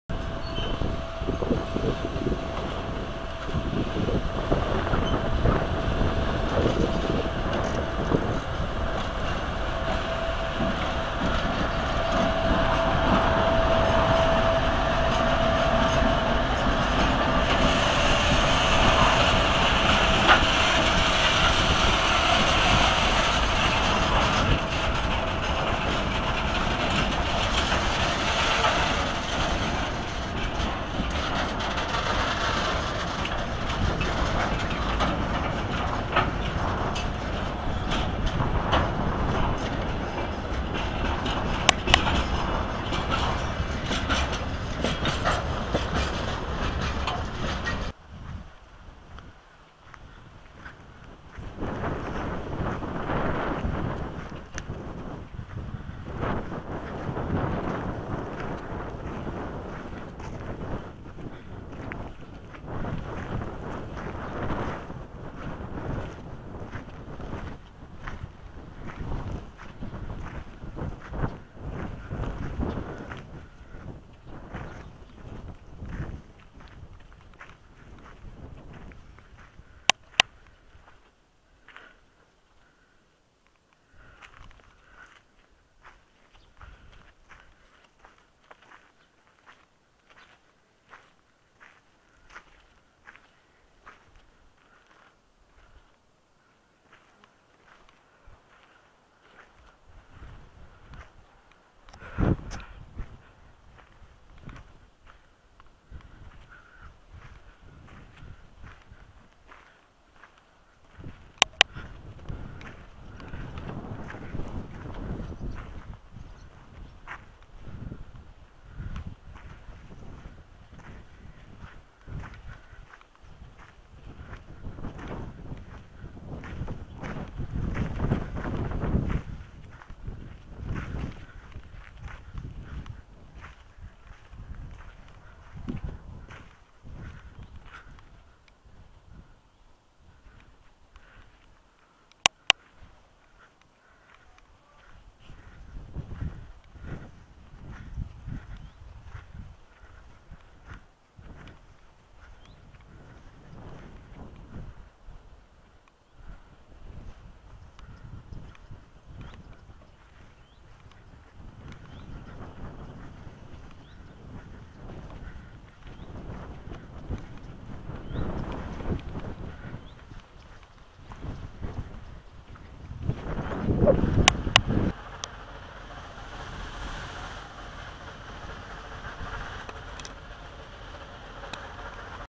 Wizyta na Stacji Siemianówka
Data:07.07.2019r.

Spis treści:
1.Białoruski M62yk-3101 z krótkim składem 0:00
2. Wagony na bocznicy 0:48
3. Ogólny widok na stacje 2:54